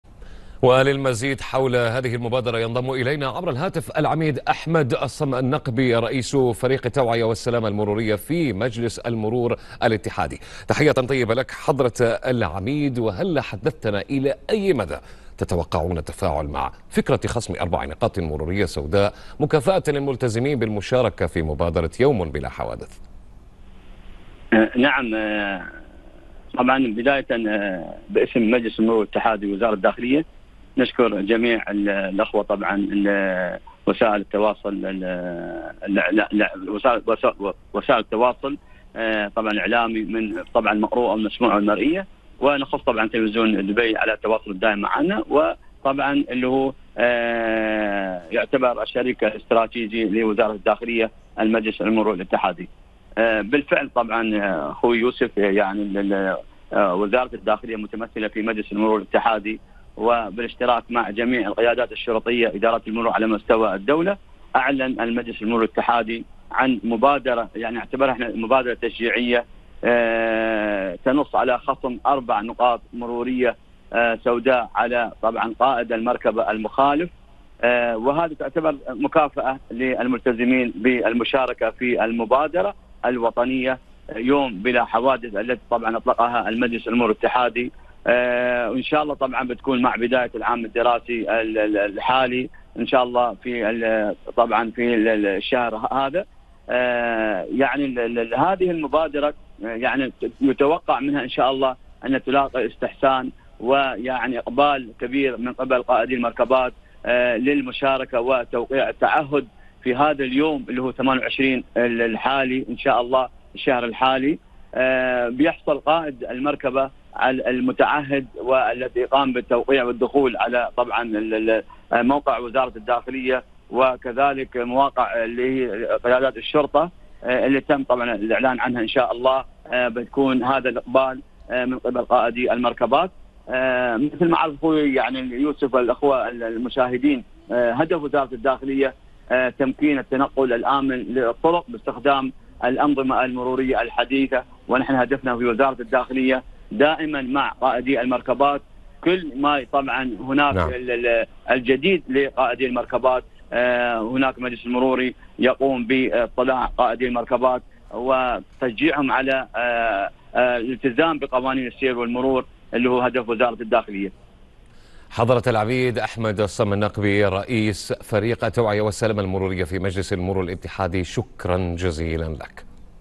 مداخلة مع العميد أحمد الصم النقبي رئيس فريق التوعية والسلامة المرورية في مجلس المرور الاتحادي
أخبار الإمارات | مداخلة مع العميد أحمد الصم النقبي رئيس فريق التوعية والسلامة المرورية في مجلس المرور الاتحادي